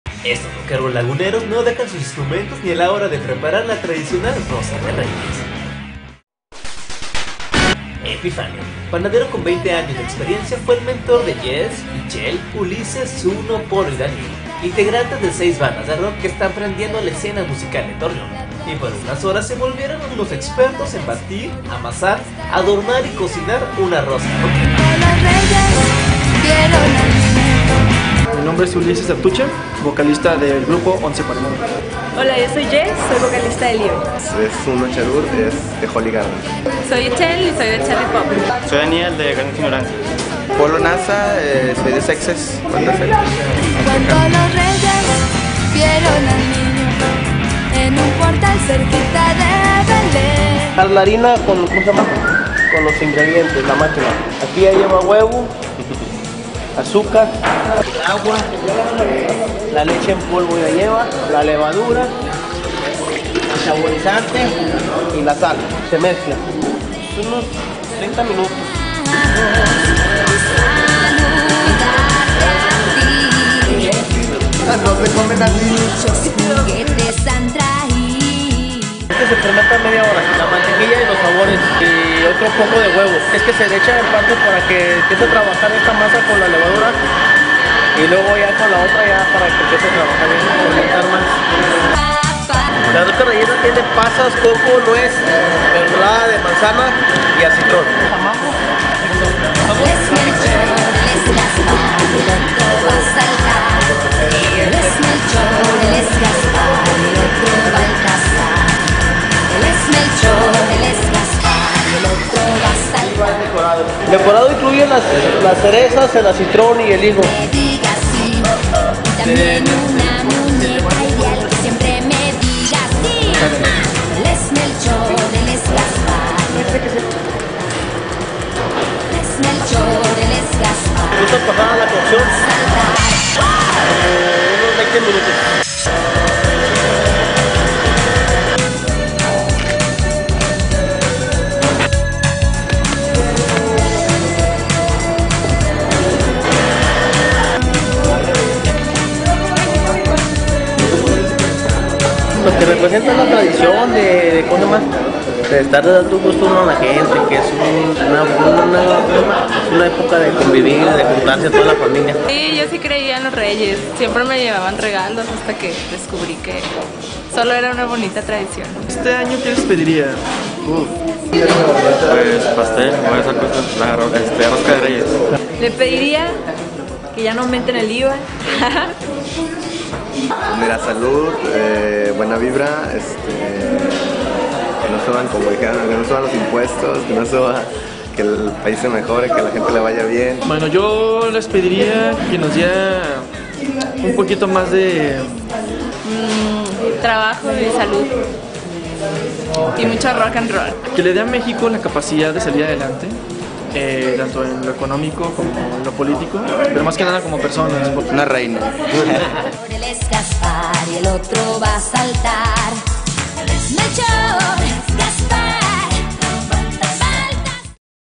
Una 'rosca rcokera':
Rockeros laguneros de diversas agrupaciones se reúnen para realizar su propia rosca de Reyes en esta especial fecha.